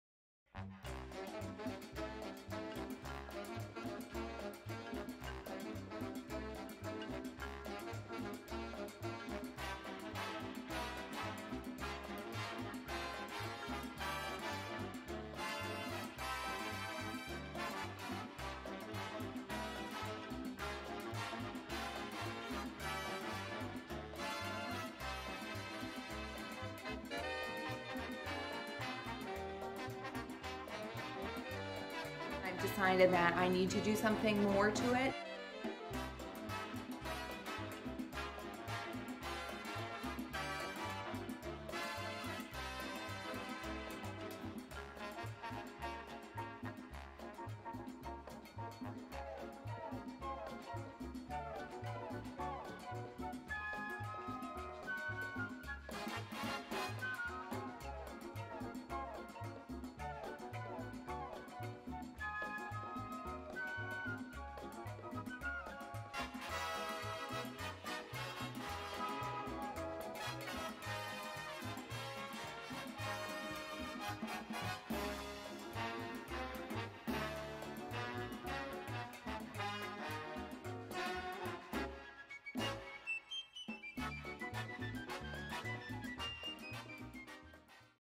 How To Decorate a Christmas Tree with Hotdogs | The Glorified Craft Show
What do you get when you combine a 12" bottle brush tree, cabochon hotdogs, and 30 sticks of high-temp hot glue? MAGICRAFTMAS.

If you're tired of having the same Christmas decorations as everyone else in your small town, because you all shop at the same Kmart, then  you need a "Hotdog, It's X-Mas" Tree!